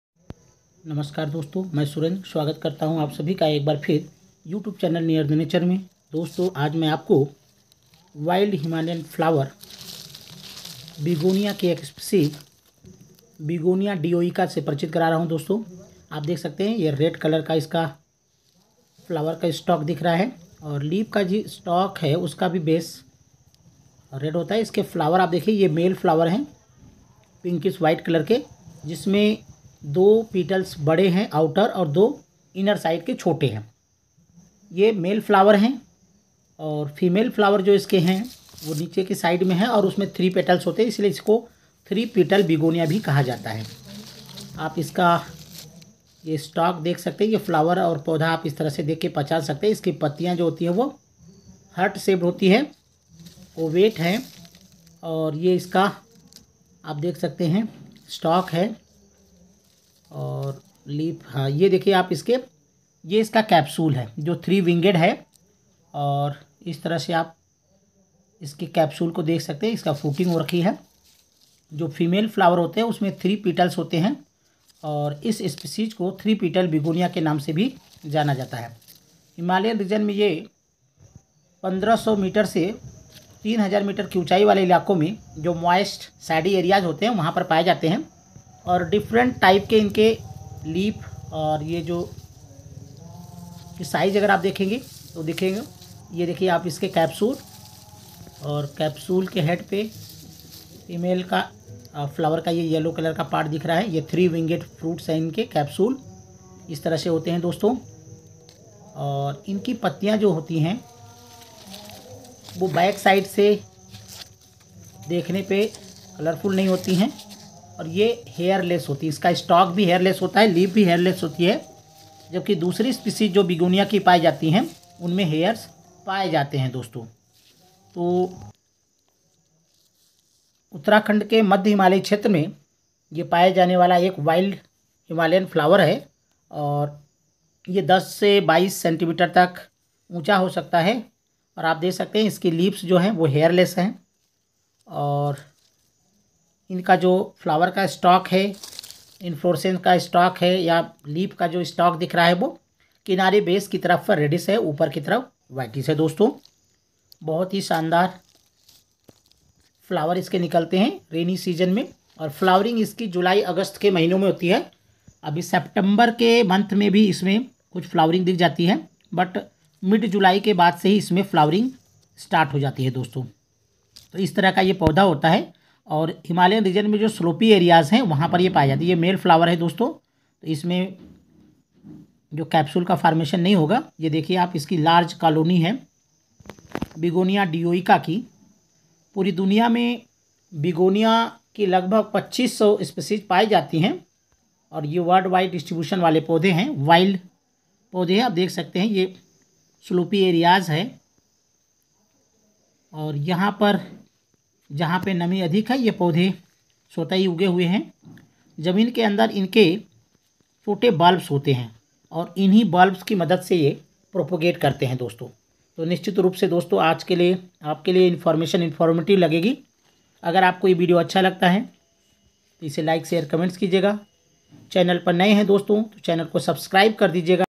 Begonia dioica
Begonia is a genera of family Begoniaceae. This genera contains 2500 species in all over world. In Himalayan region of Uttarakhand it one species Begonia dioica is very common at the height between 1500 meter to 2700 meter. It is found in moist, shady, sloppy area. Its leaves are heart shaped without hairs, Leaf and flower stack also hairless. Its fruits are capsule and three winged. Its leaves and flower stack base red in colour and looking very attractive. It  propogates through bulbs.